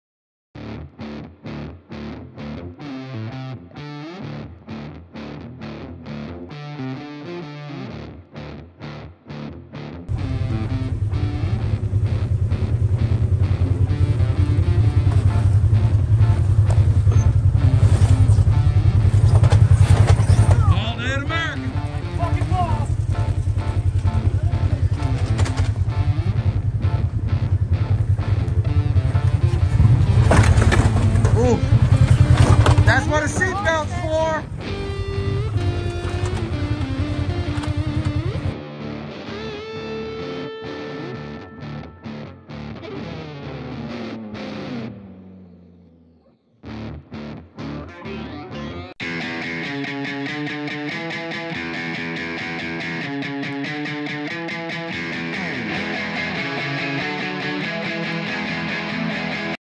Seatbelt Fail-This Is Why You Wear A Seatbelt And Helmet
Just a short from this last trip we took to hatfield mccoy trails in rock wv